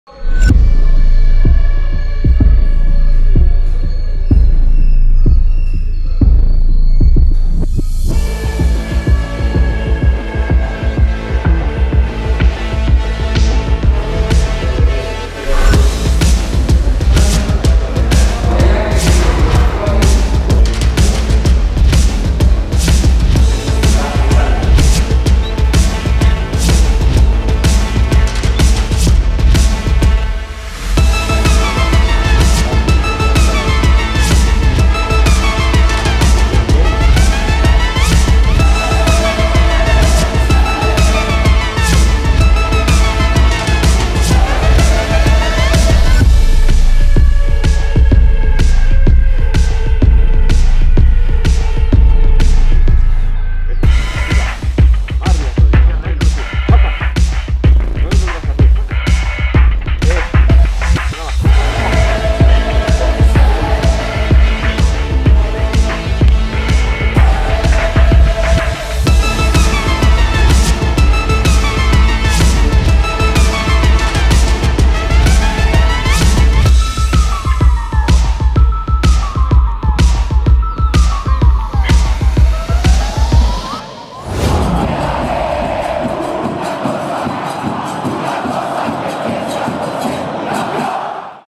¡El trabajo no para! Día de Gimnasio
El compromiso de los Guerreros Albos no flaquea, con la mente puesta en el encuentro frente a Universidad Católica se ejercitaron este miércoles en el Gimnasio del CAR Herbalife de Pomasqui mientras los arqueros albos realizaron trabajos de fortalecimiento y agilidad en cancha.

Subscríbete en nuestro canal oficial de Youtube para más contenido.